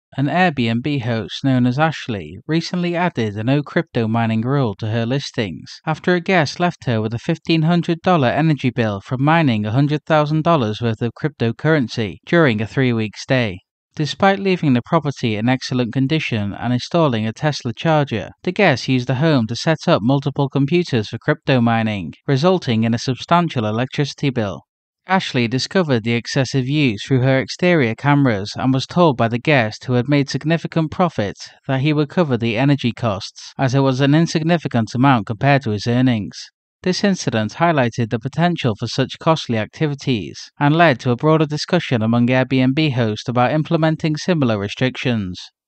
$1500 Airbnb Electricity Bill Due To Crypto Mining
An Airbnb host in the U.S. had to add a "no crypto mining" rule after a guest racked up a $1,500 energy bill by mining $100,000 in cryptocurrency during a three-week stay. The guest left the property in excellent condition but caused significant power consumption with multiple computers. This incident has prompted other Airbnb hosts to consider similar restrictions.